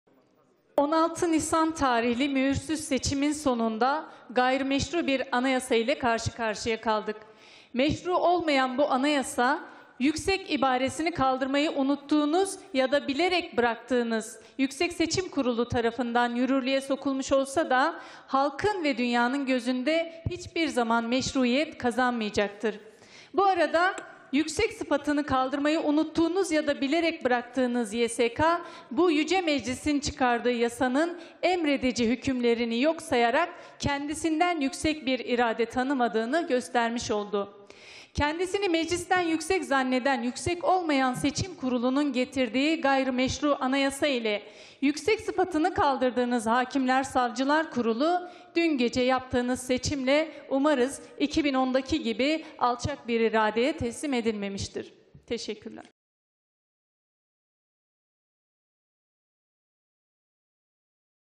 Umarız 2010'daki Hataya Düşmemişsinizdir!
Bursa Milletvekilimiz Nurhayat Altaca Kayışoğlu: 16 Nisan tarihli mühürsüz seçimin sonunda, gayrimeşru bir anayasayla karşı karşıya kaldık. Getirdiğiniz bu gayrimeşru anayasayla "yüksek" sıfatını kaldırdığınız Hâkimler Savcılar Kurulu, dün gece yaptığınız seçimle umarız 2010'daki gibi alçak bir iradeye teslim edilmemiştir!